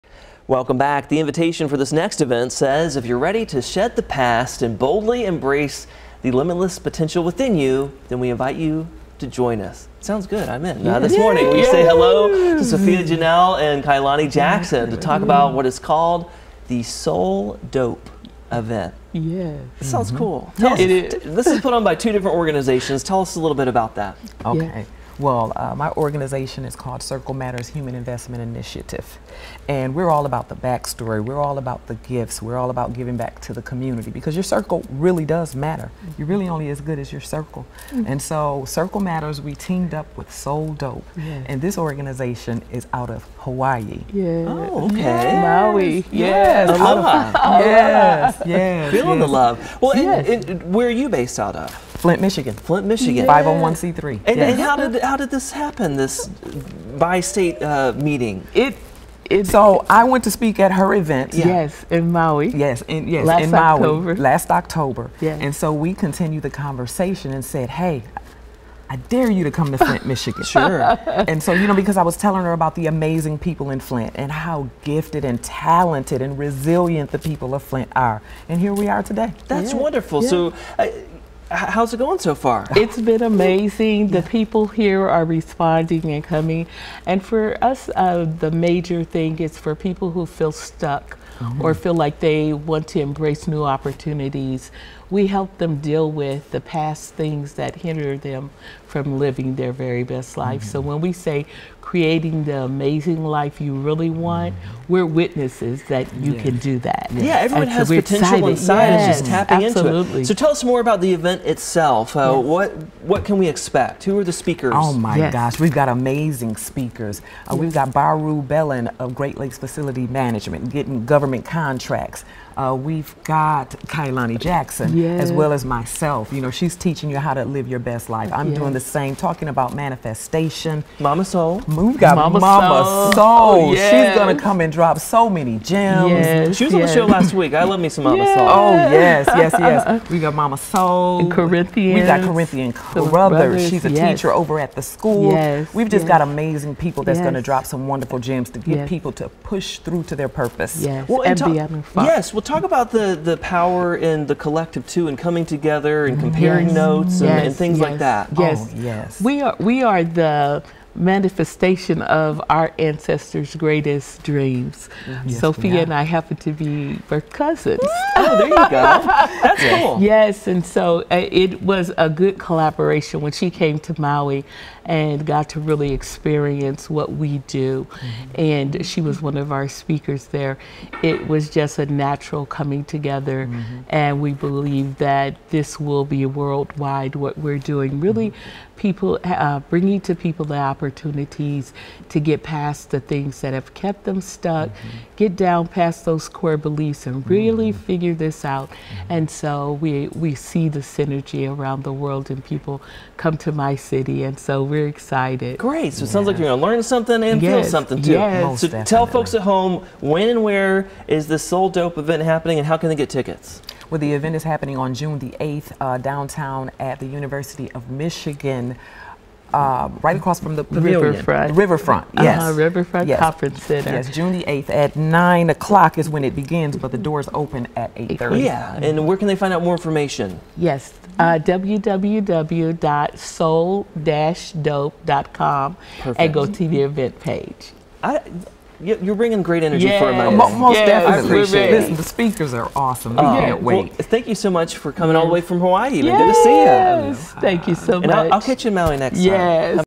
"Soul Dope" event in Flint to have inspirational panel of speakers
Soul Dope & Circle Matters say, "If you're ready to shed the past and boldly embrace the limitless potential within you, then we invite you to join us."